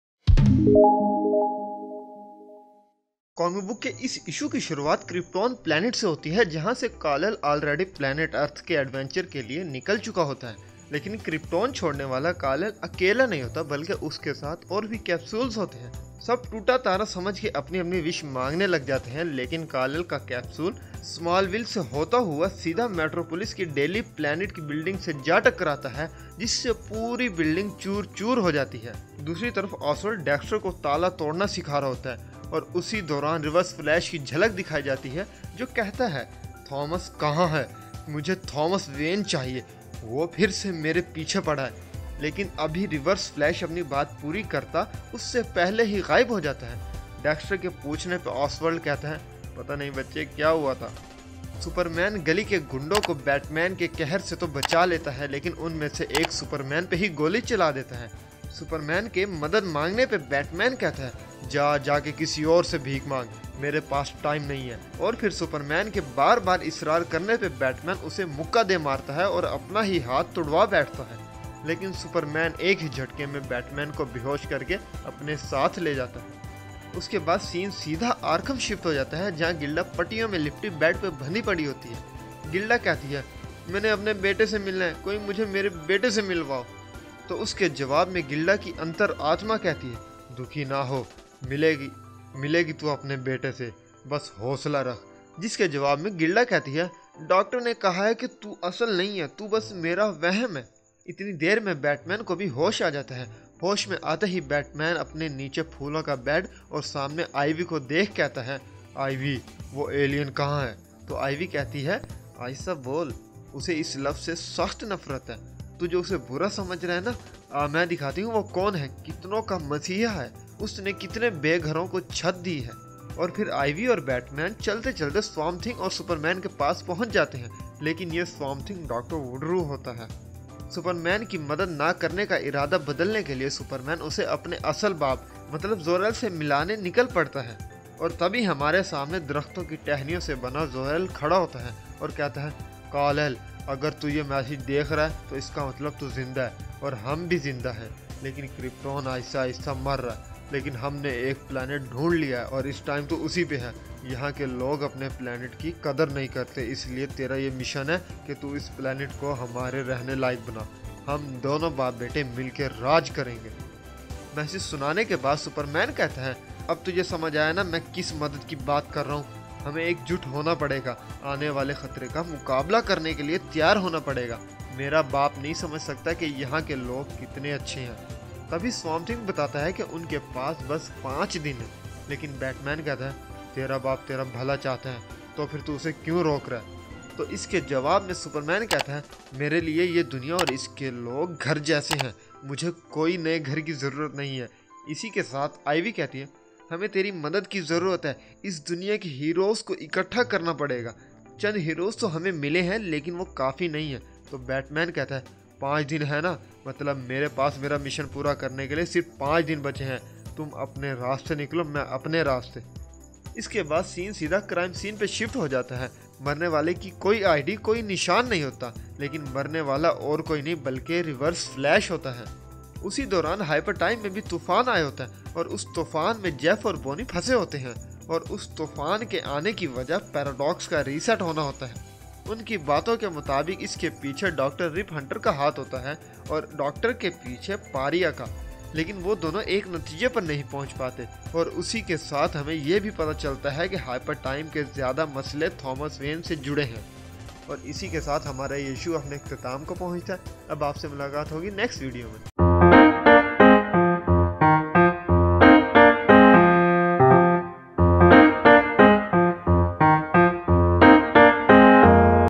Flash Point Beyond | Issue #3 | Urdu/Hindi
Flash Point Beyond is the comic book from DC comics which tells the story after the Flash Point Event. Thomas Wayne's Batman is willing to correct everything all over again, and doing everything to make it right, either its right or wrong.

Flash Point Beyond issue #3 narrated in Urdu/Hindi language for the persons who don't have access to comics books or don't have enough time to read them.

To Comic Core Superhero Core.